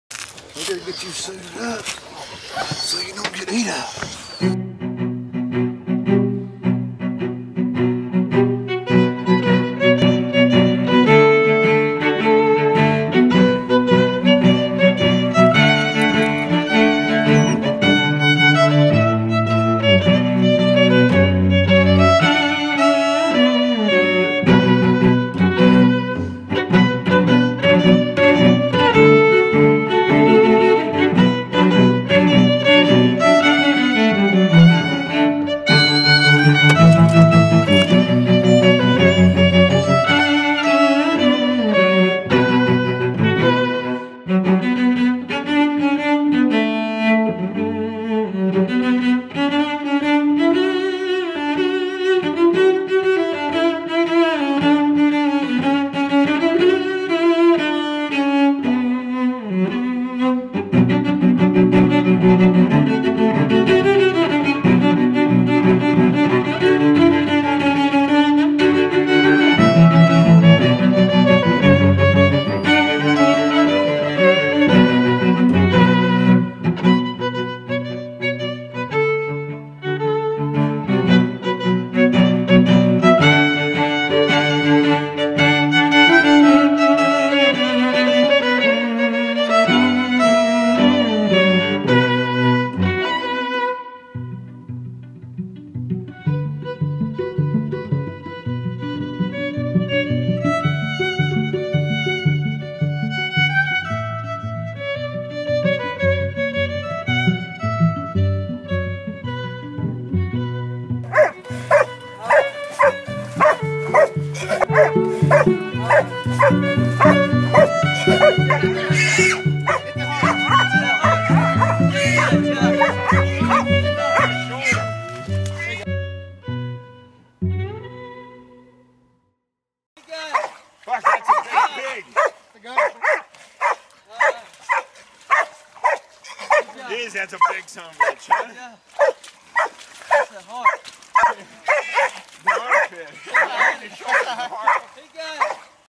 Adventure Time Hunting Arkansas Razorbacks in the South with Sparks Worldwide
Adventure Time   Hunting Arkansas Razorbacks in the South with Sparks Worldwide. A tribute to my father. Found some old footage from the last time Pops and I hunted together. This is my first video using Premiere Pro. Used a bunch of iphones and a Canon powershoot.